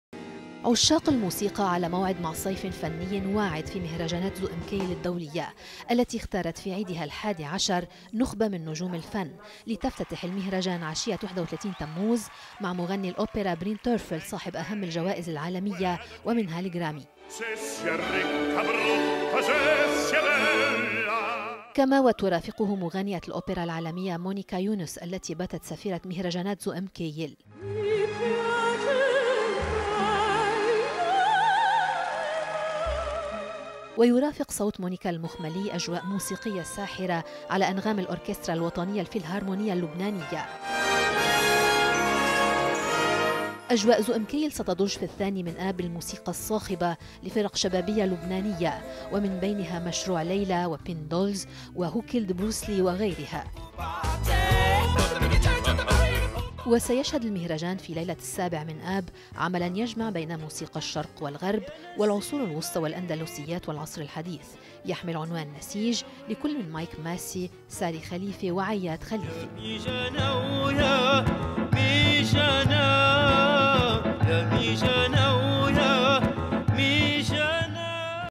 LBCI News-صيف فني واعد في مهرجانات الزوق
عشاق الموسيقى على موعد مع صيف فني واعد في مهرجانات  زوق مكايل الدولية